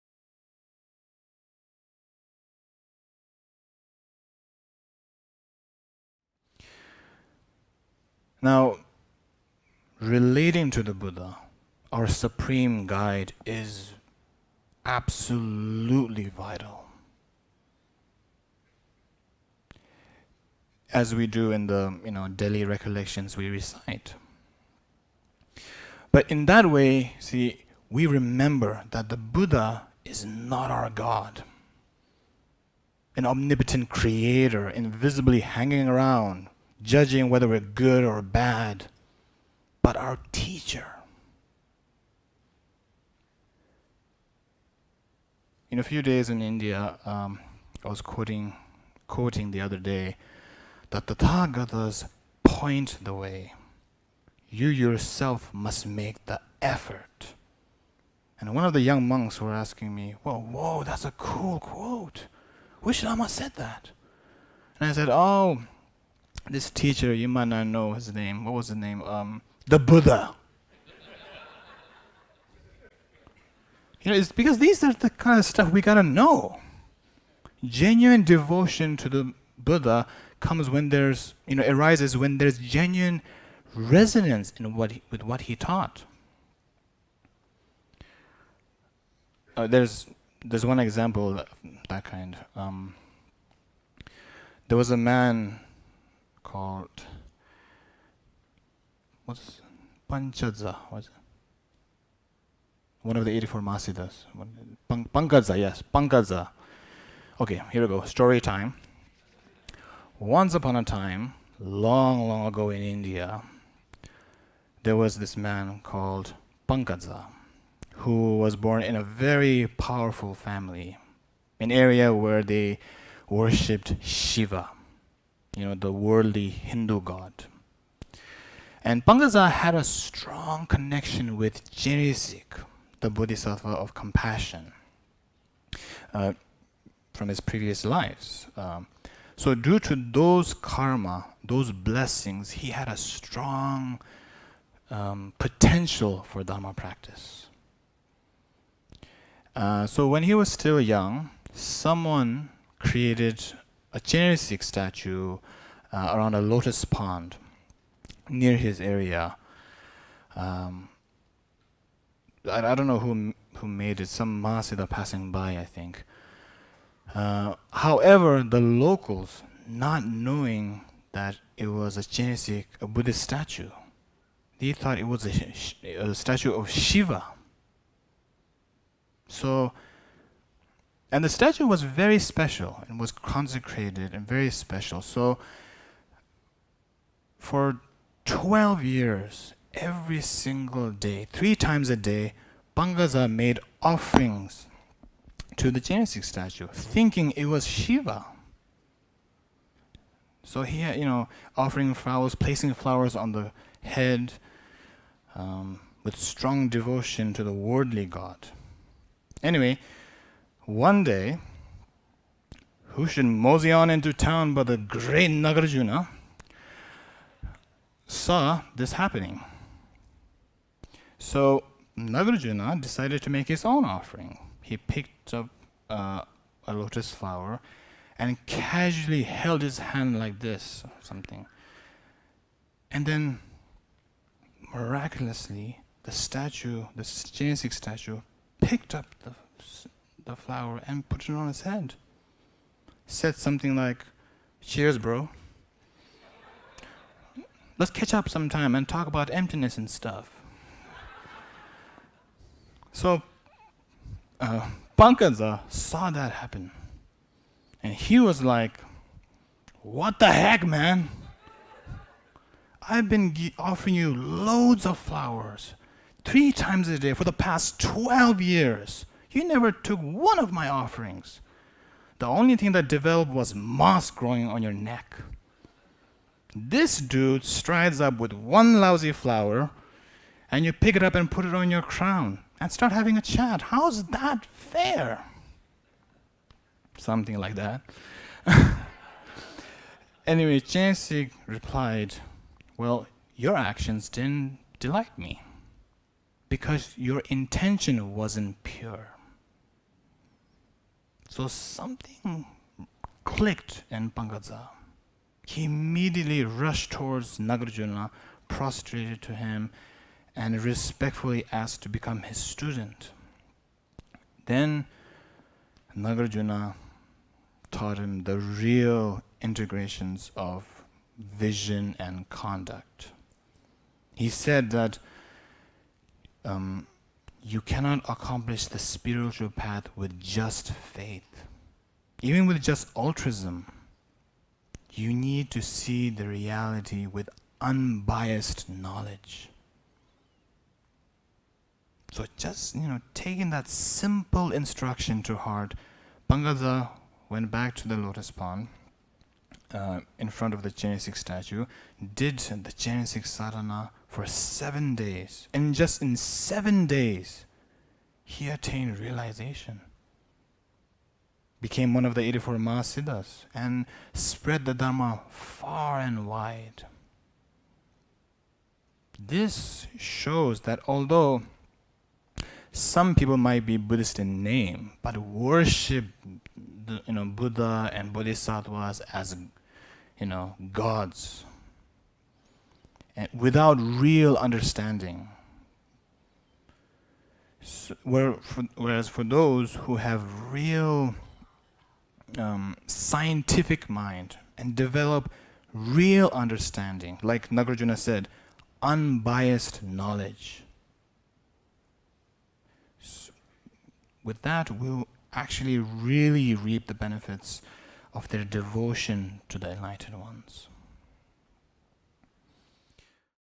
Unbiased Knowledge - Avikrita Vajra Rinpoche
In this short clip from Six Dharmas programme in Seattle in February 2017, Rinpoche tells the story of Nagarjuna's encounter with a young Mahasiddha in the making as an encouragement for us to discover unbiased knowledge in our spiritual quest.